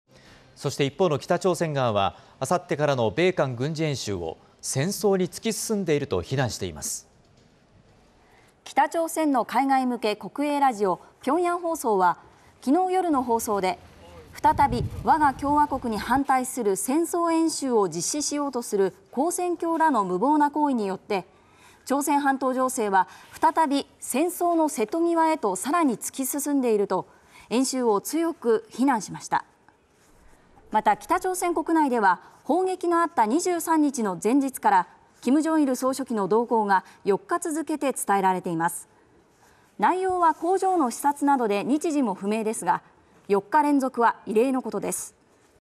【北砲撃】北朝鮮「戦争に突き進んでいる」と報道（10/11/26）
北朝鮮側は、28日からの米韓軍事演習を「戦争に突き進んでいる」と非難しています。